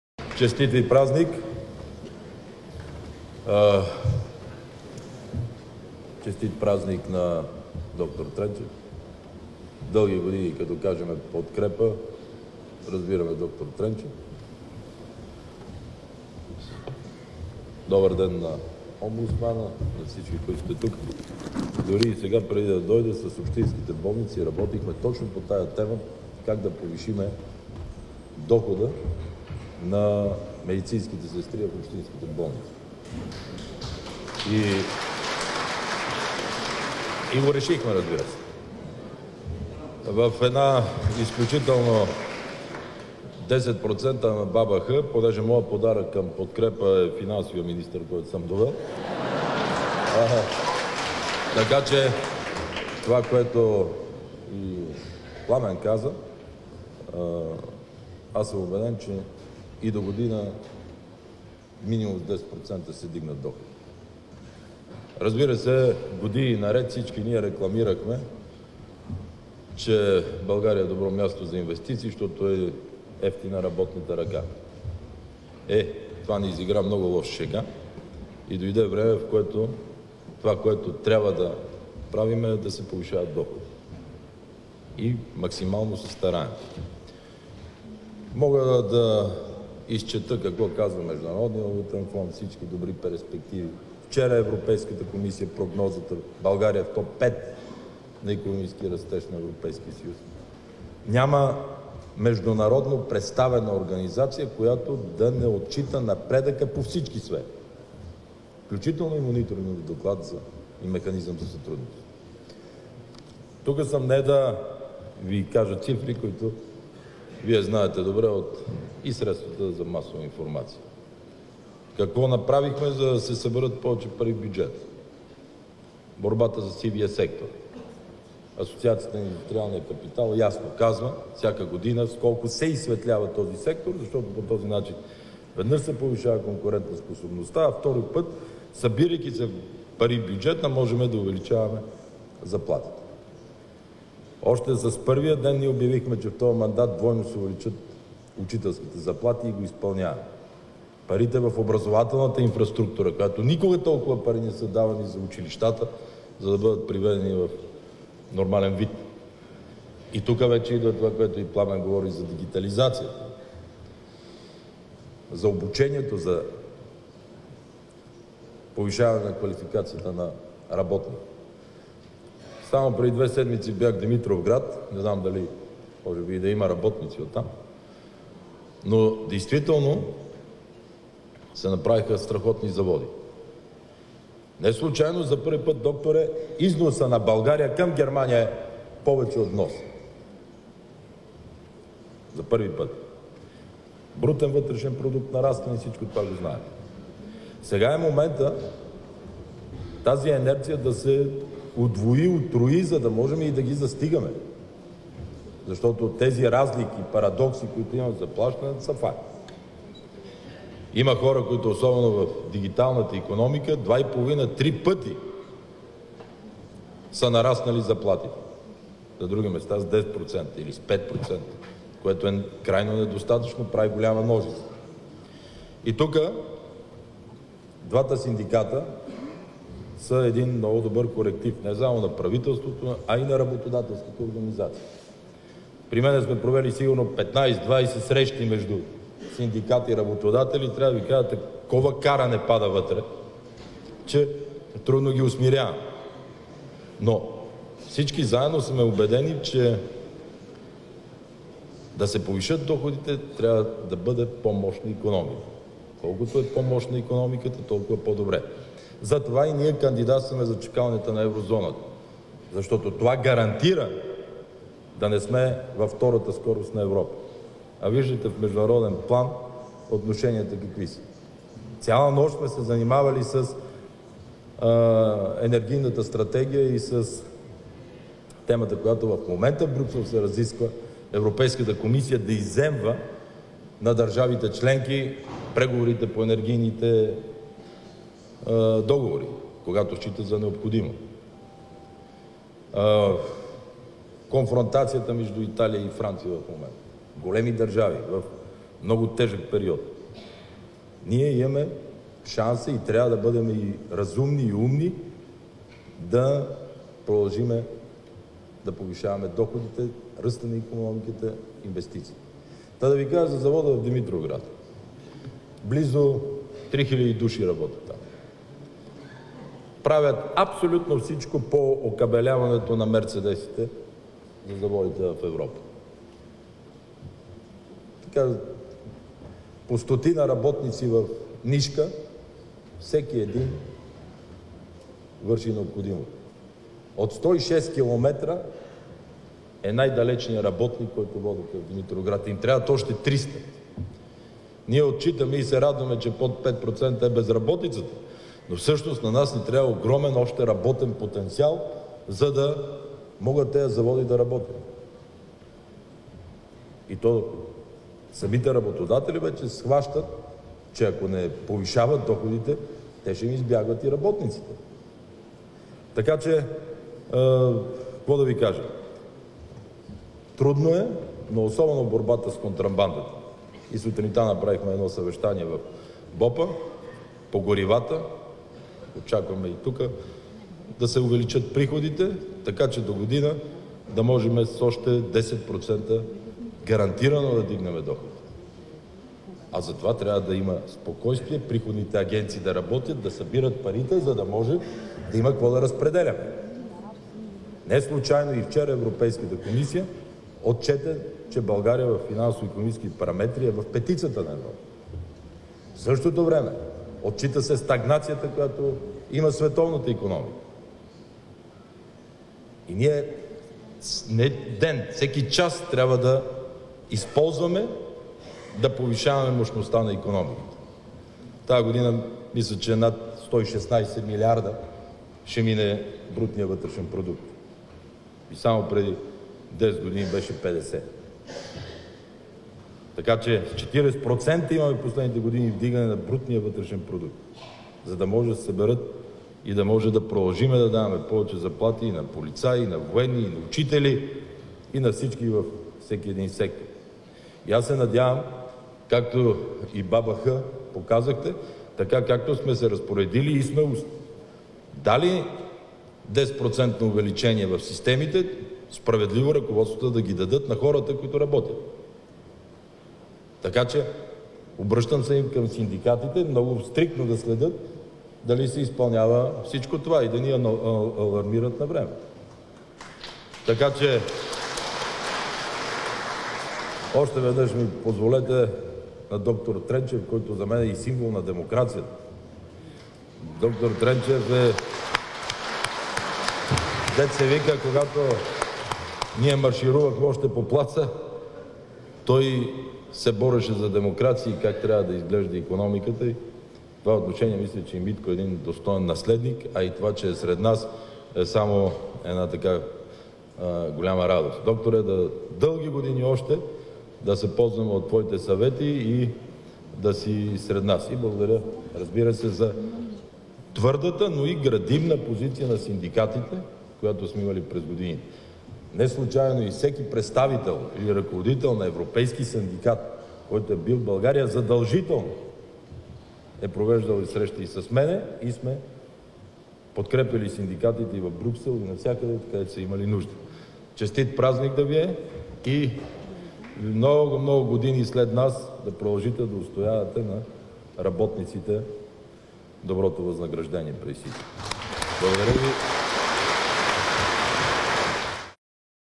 Бойко Борисов - 10% ръст на доходите догодина
"Вдигането на заплатите е неизбежно. При дефицита на работна ръка пазарът го изисква", от своя страна заяви министърът на финансите Владислав Горанов.
Той пожела на делегатите здраве и да изберат за лидер правилния човек, когото той "само погледна" - сегашния президент на профсъюза Димитър Манолов, който преди 3 г. пое щафетата от почетния лидер д-р Тренчев.
На конгреса са още президентът Румен Радев, кметът на София Йорданка Фандъкова, омбудсманът Мая Манолова, вицепремиерът Марияна Николова, социалният министър Бисер Петков.
Малко над 2 милиона домакинства в България нямат нито едно дете, само 300 хиляди са с две или повече - такава статистика прозвуча от трибуната.
За 3 г. да върнем 100 000 българи от Молдова и Украйна, за да променим демографията, ни трябват само 200 милиона лева ще на година, а ние харчим милиарди за глупости, подчерта от трибуната лидерът на КНСБ Пламен Димитров. За 4 г. може да постигнем средните доходи за Европа - такъв разчет направил той.

1.7 млн. сега са под прага на бедност, подчерта той.
8.2 пъти е разликата между най-високата и най-ниската заплата в страната, а в отделни случаи разликата надхвърля 100 пъти.